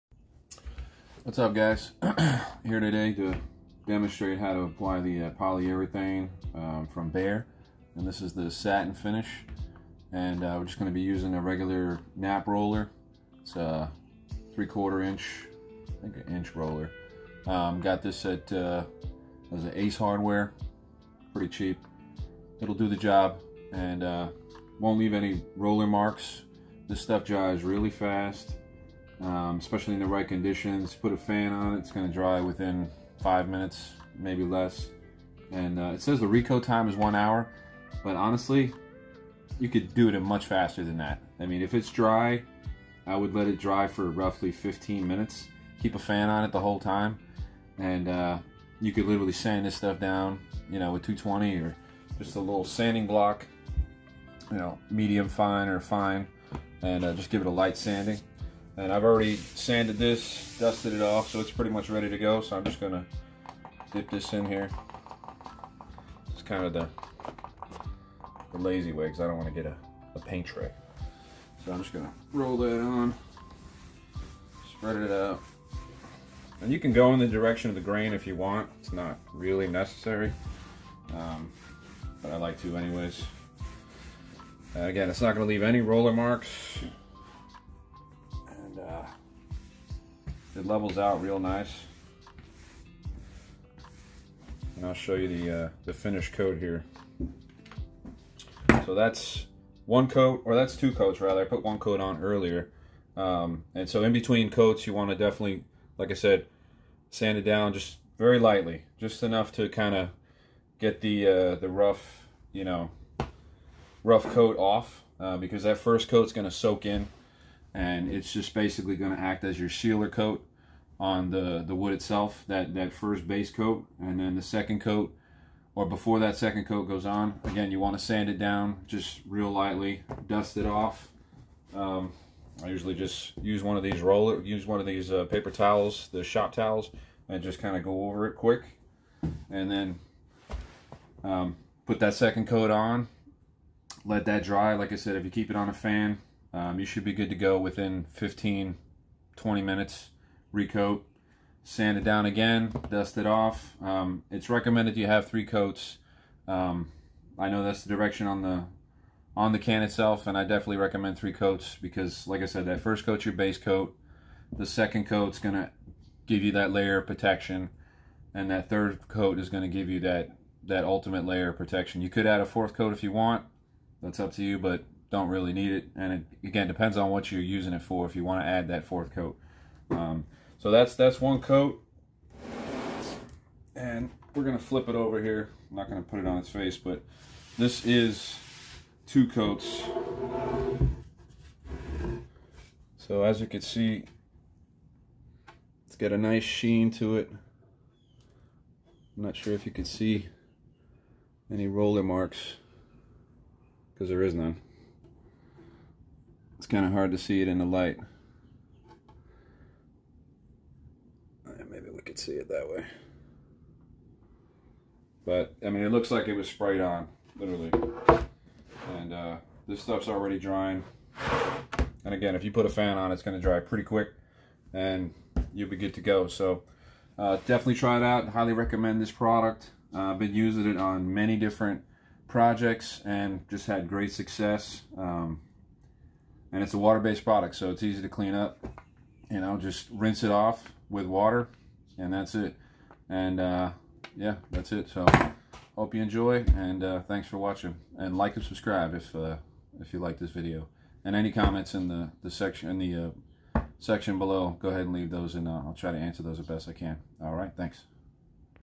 How to apply water-based polyurethane from Behr
In this video, you'll see my quick and easy methods to apply this fantastic water-based poly.